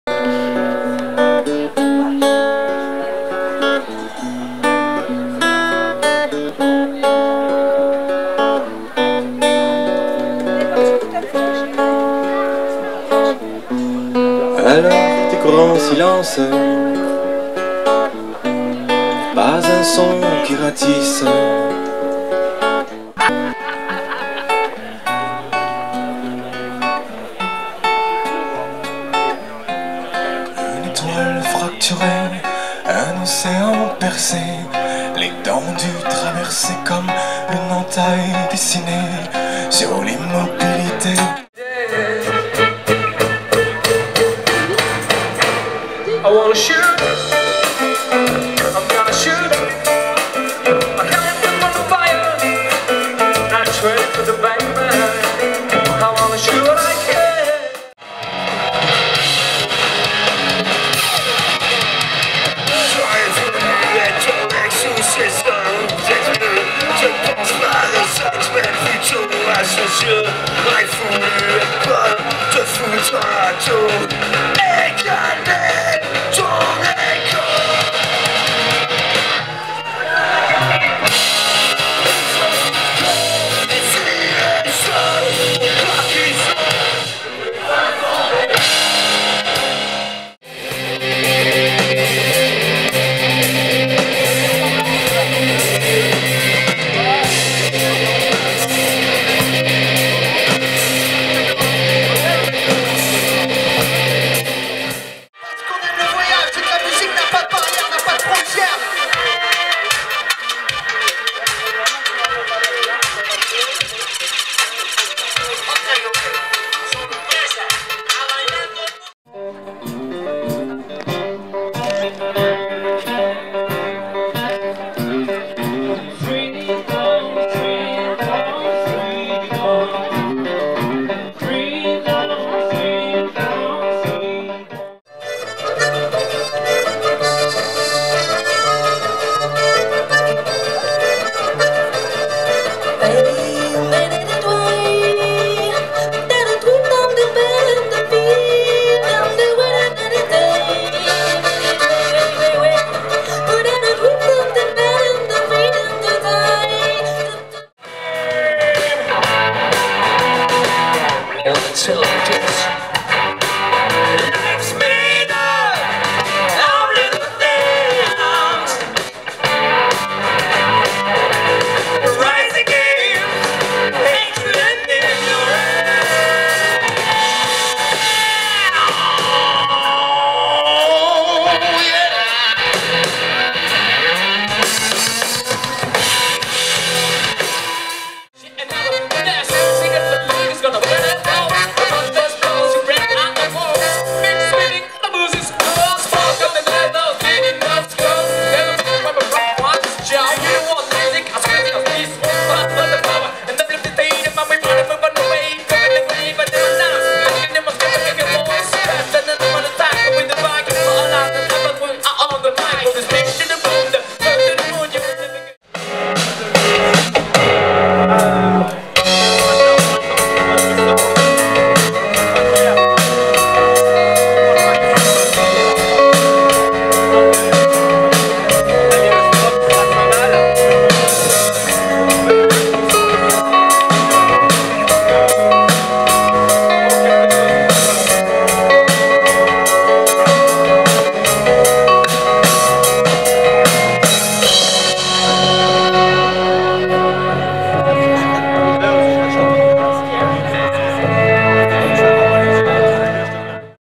k-cow phonies 2017
notre petit after movie sur les k-cow phonies, des gens super on a passé un excellent moment avec vous